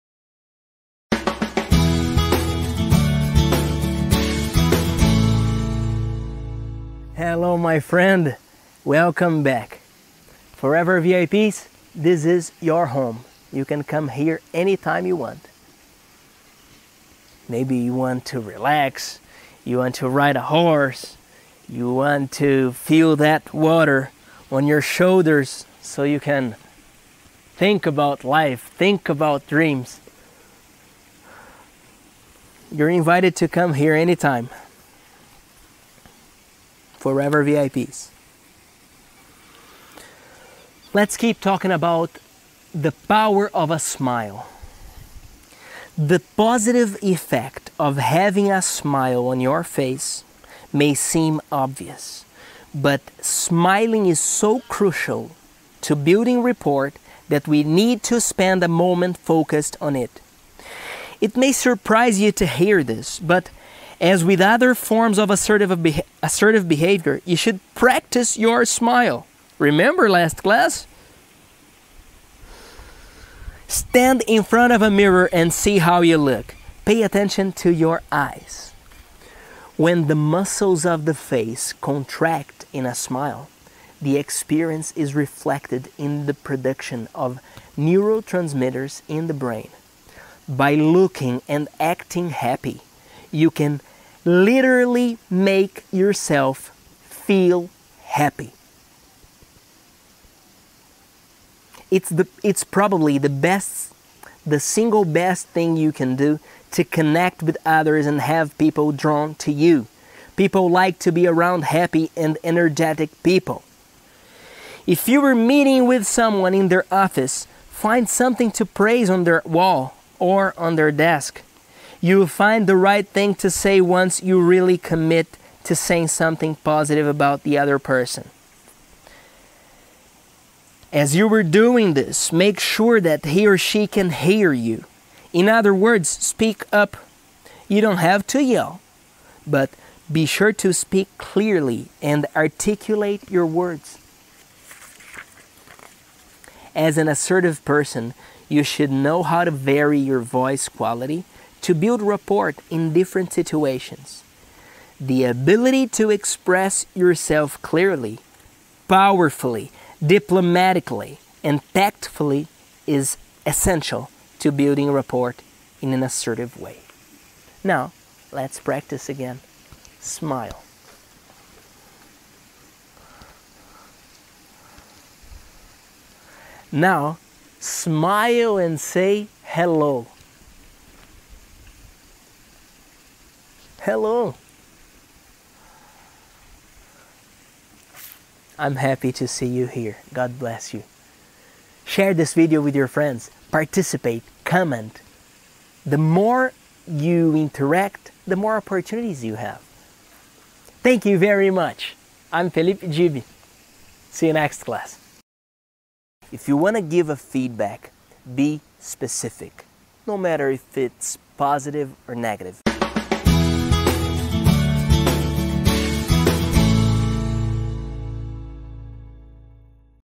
15- Should I smile and speak up?
Clase del curso de ingles y liderazgo tu aprendes ahora the 5 essential people skills, written by Dale Carnegie.

Hello, my friend! How are you? If you're out of time or "in a rush", go to the last paragraph of this text for a fast answer.

In times of immediate responses and connection 24 hours a day, talking about long-term transformation is a risk! But… we believe you can (and must!) dream big, and make a daily effort to get better every day. This is our mission - seeing you speaking English and being a Leader wherever you go. "The 5 Essential People Skills" was written by Dale Carnegie and its goal is to improve your people skills. In other words, to improve your relationships at work and at home.

Before we start studying, smile. Smile, my friend! Are you smiling with your mouth shut? Open your mouth, take a deep breath and smile…

Good! I love to see you smiling! "The positive effect of having a smile on your face may seem obvious, but smiling is so crucial to building rapport that we need to spend a moment focused on it". I know my next words might sound weird, but you should practice your smile. Stand in front of a mirror and see how you look. Pay attention to your eyes, because they're the ones who will reveal a real smile.

There's science behind your smile, and "when the muscles of the face contract in a smile, the experience is reflected in the production of neurotransmitters in the brain". Carnegie explains that by looking and acting happy, you can literally make yourself feel happy.

Another important factor when you communicate is your intonation. Make sure people can hear you clearly when you talk. "As an assertive person, you should know how to vary your voice quality to build rapport in different situations".

Smile and speak up, but do it with joy. Practice your smile and always check your eyes to see if you're really smiling. Don't yell, but articulate your words with energy so people can hear and understand your message. How often do you smile? Comment here and see you next class!